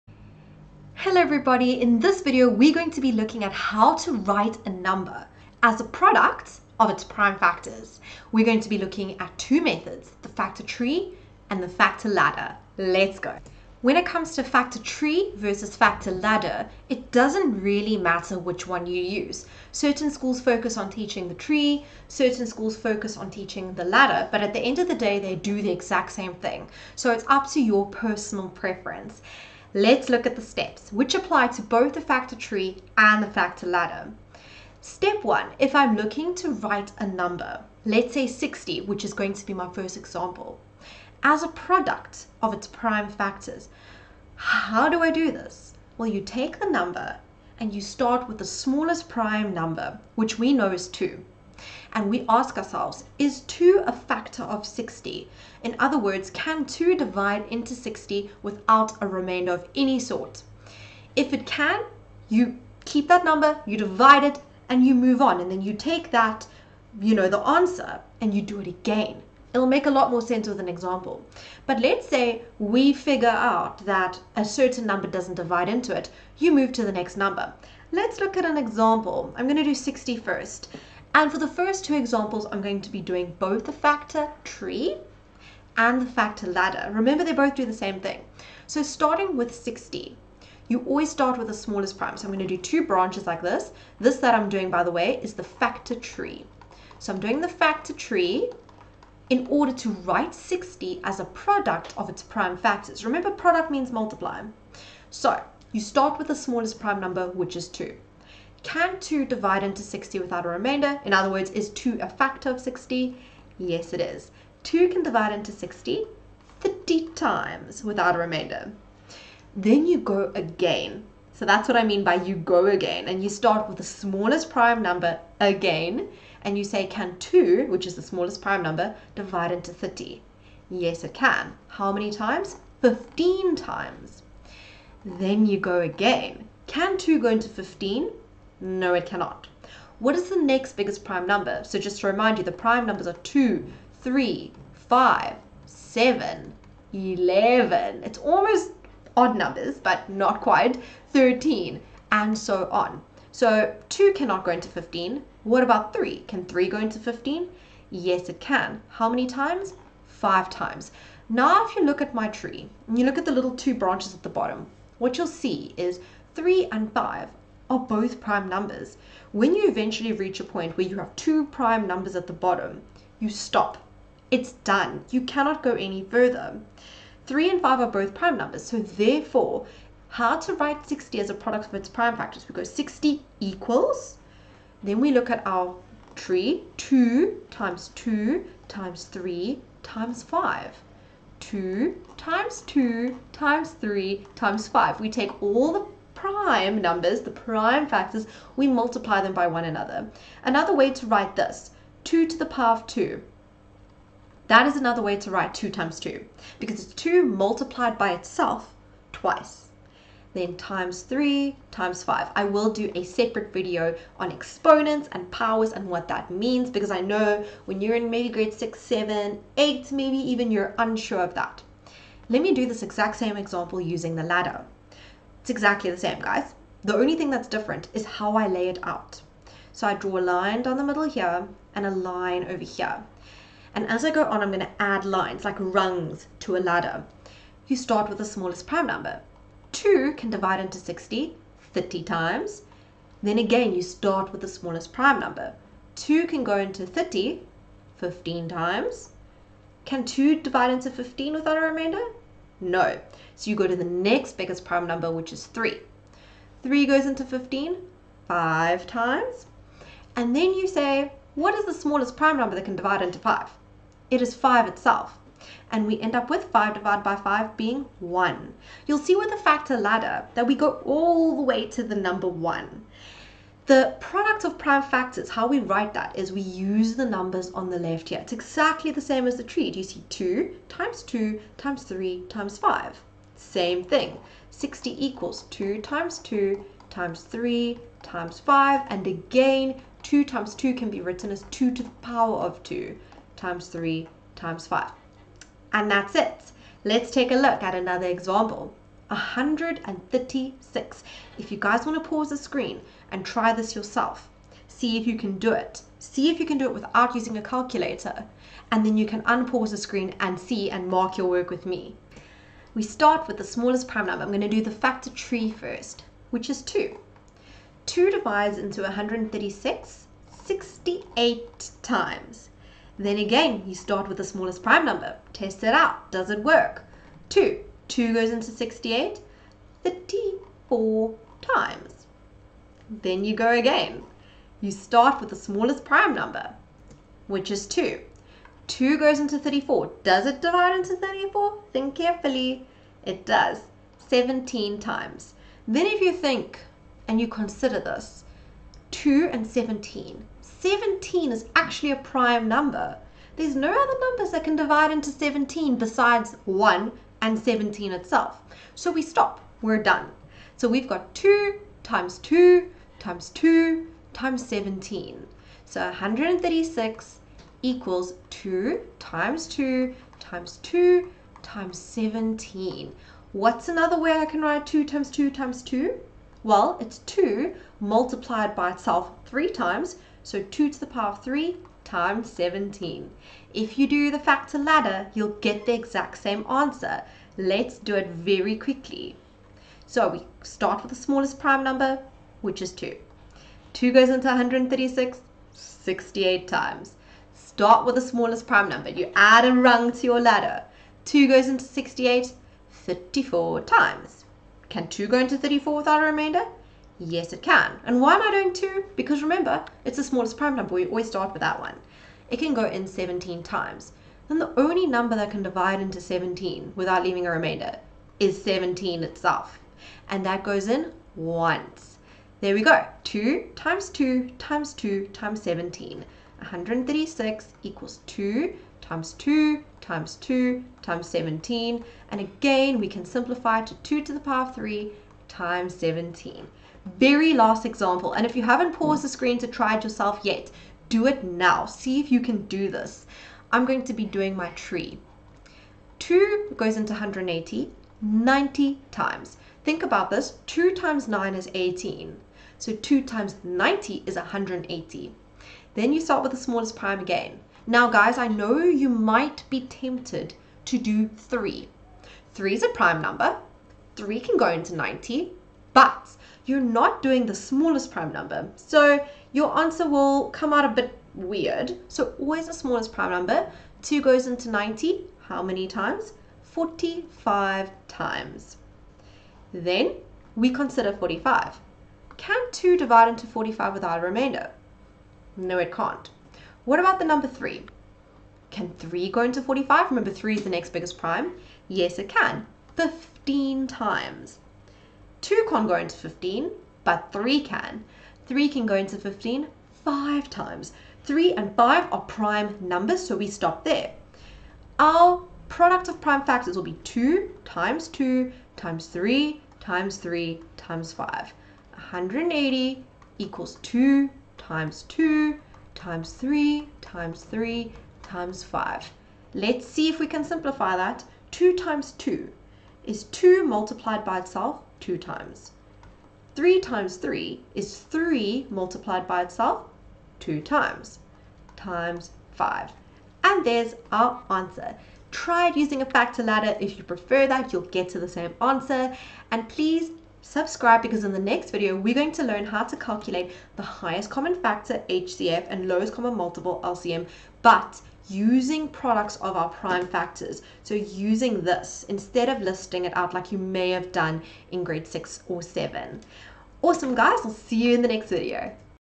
Writing a number as a product of its prime factors using the factor tree or ladder
GRADE 7, 8 AND 9 MATHS
In this video I will show you how to write a number as a product of its prime factors using a FACTOR TREE OR A FACTOR LADDER!

(In videos to come I will go over how to calculate the highest common factor, lowest common multiple, how to simplify fractions using PRIME FACTORS)